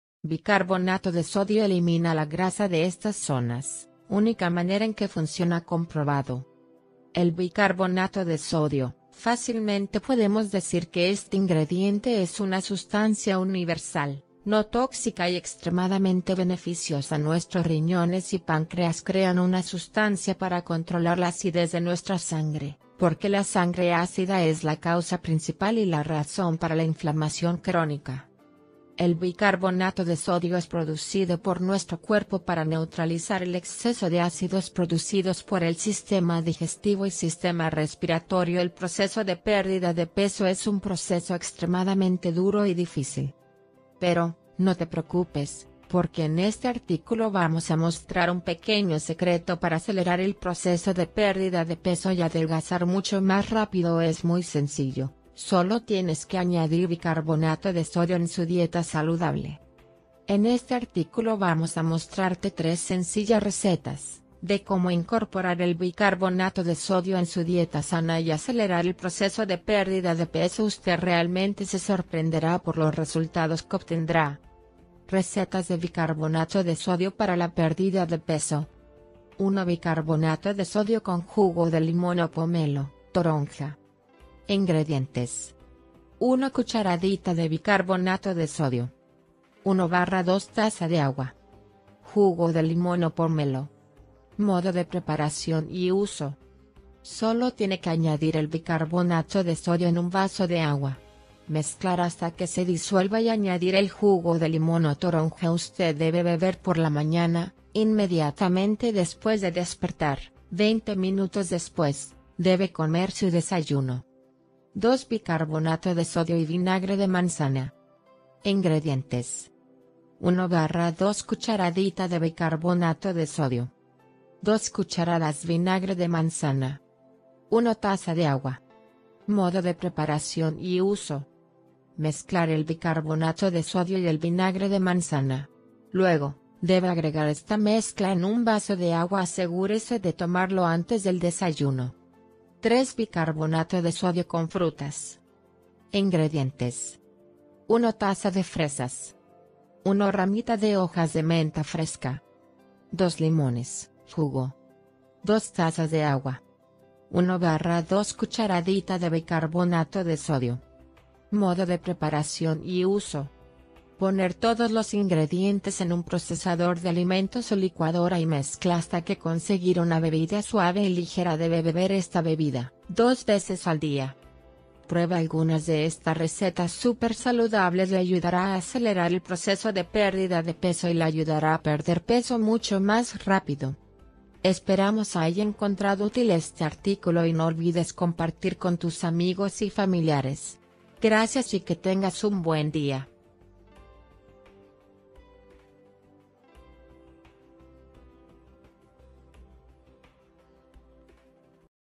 Bicarbonato de sodio elimina la grasa de estas zonas: única manera en que funciona COMPROBADO!
El bicarbonato de sodio, fácilmente podemos decir que este ingrediente es una sustancia universal, no tóxica y extremadamente beneficiosa. Nuestros riñones y páncreas crean una sustancia para controlar la acidez de nuestra sangre, porque la sangre ácida es la causa principal y la razón para la inflamación crónica.